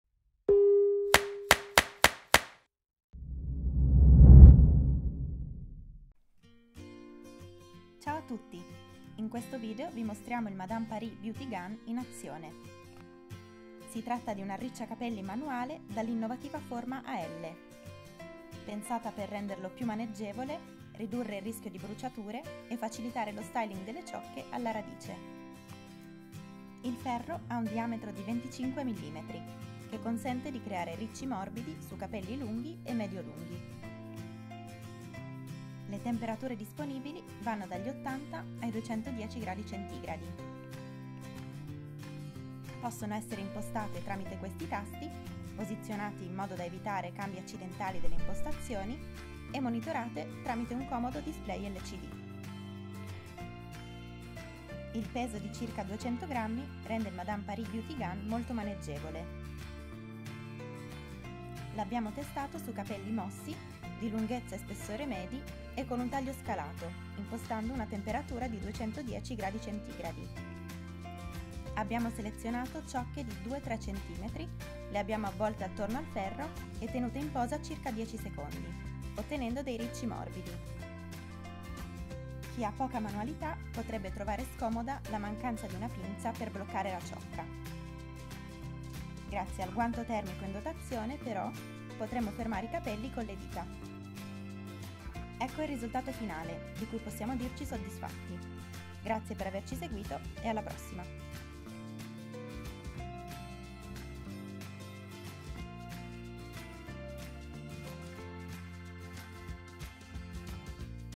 Madame Paris Beauty Gun in azione | QualeScegliere.it
🔻  🔻  Clicca qui sotto per ulteriori informazioni 🔻  🔻

In questo video potete vedere l'arricciacapelli Madame Paris Beauty Gun in azione.
Consultate la nostra recensione completa se volete scoprire tutte le caratteristiche di questo modello.

Avete dubbi, domande, richieste per un prossimo video?

Grazie per averci seguiti, alla prossima!

Rimaniamo in contatto!